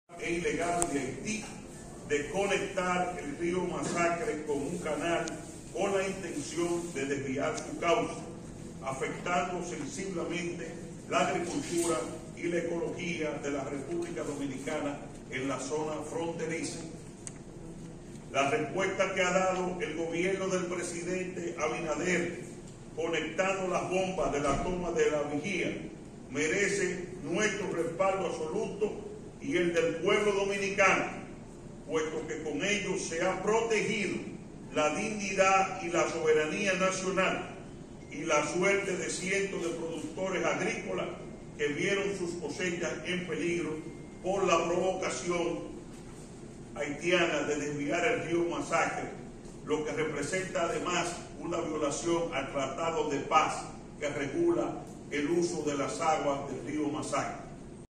Vinicio Castillo Semán, respaldan respuesta del gobierno en conflicto con Haití
Nacionales I El candidato a senador del Distrito Nacional por la alianza PRSC-FNP, Vinicio Castillo Semán, junto a otros candidatos congresuales, respaldaron la respuesta del gobierno en el conflicto con Haití por el desvío del canal del Río Masacre.